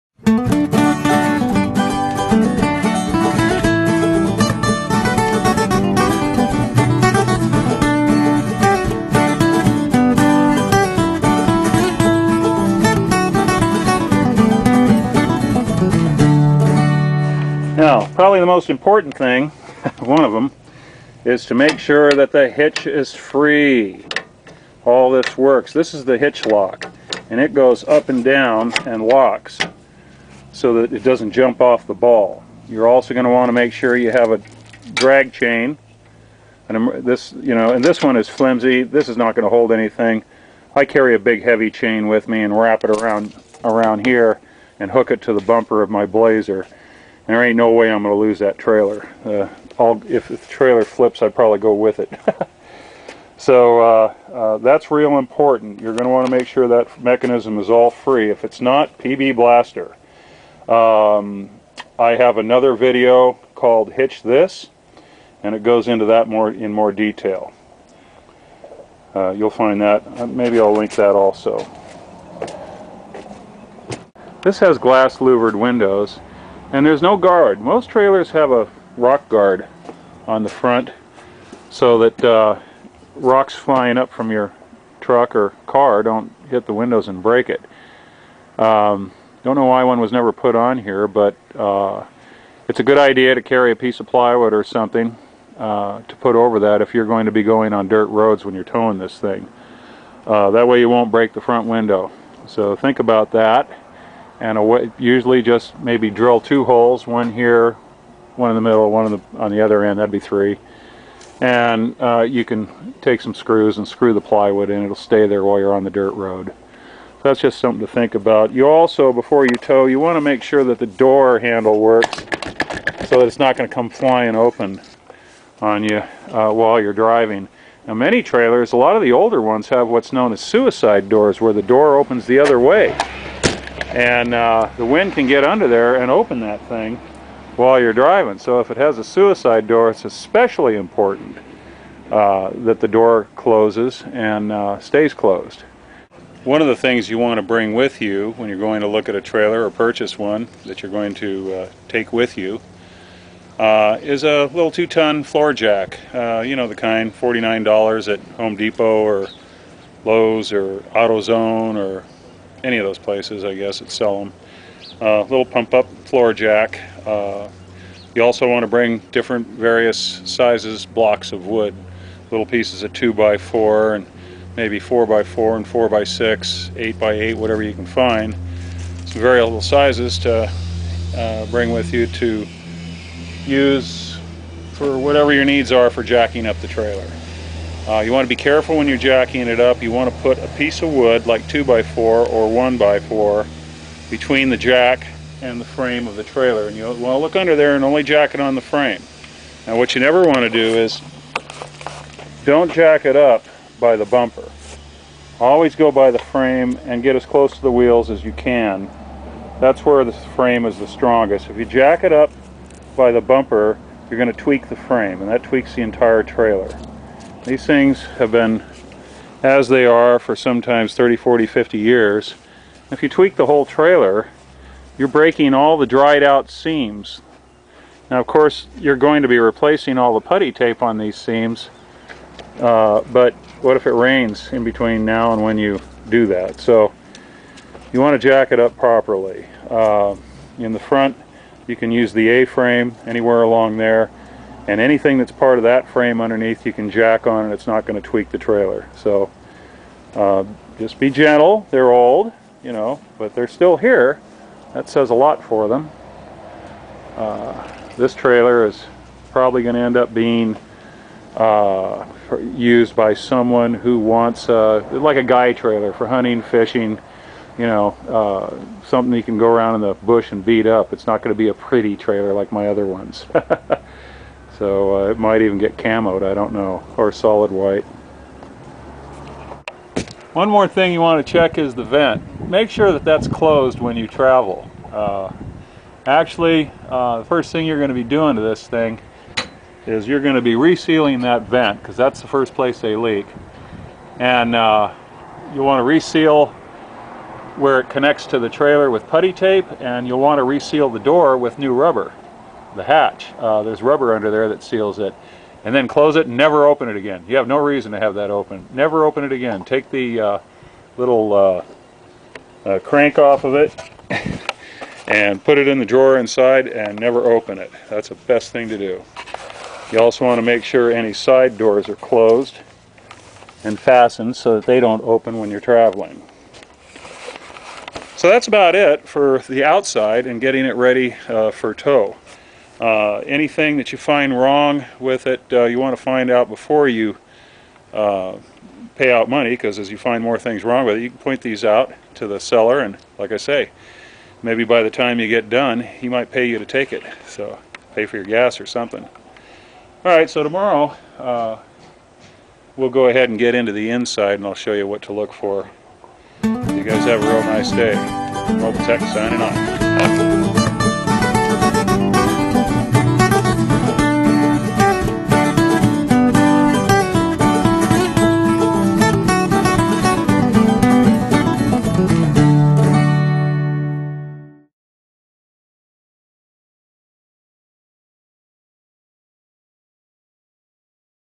Trailer Questions Part 1b
Part 1b in a series about what to look for on the exterior of a trailer before purchase or hauling. Many of the parts needed to restore or repair anything on or in your trailer can be purchased

More about hitches can be seen in my video "Hitch This".